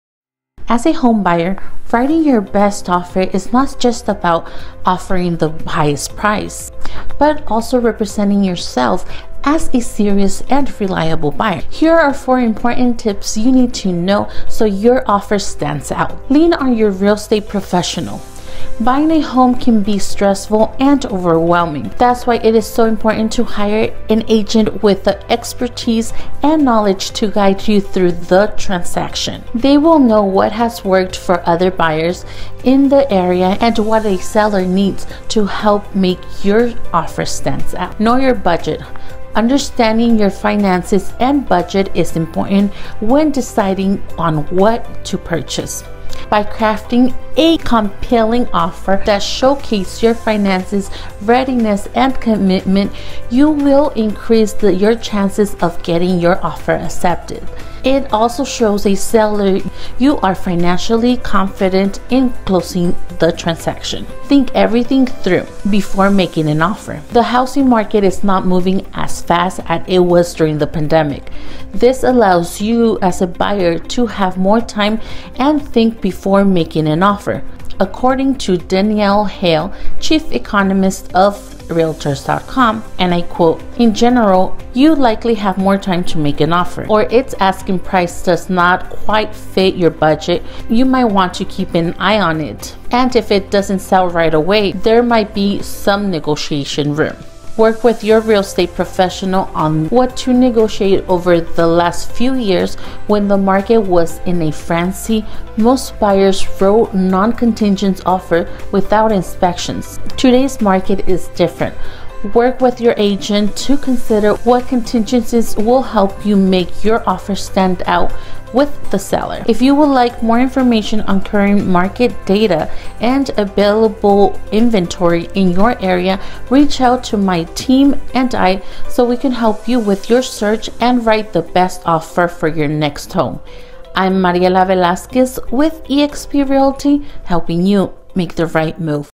Get noticed: How to stand out as a buyer and get your offer accepted.
Here is the national market update. Whether you are looking to find a larger home, find better school district or move out of the area, I am here to help.

Mariela Velazquez | Realtor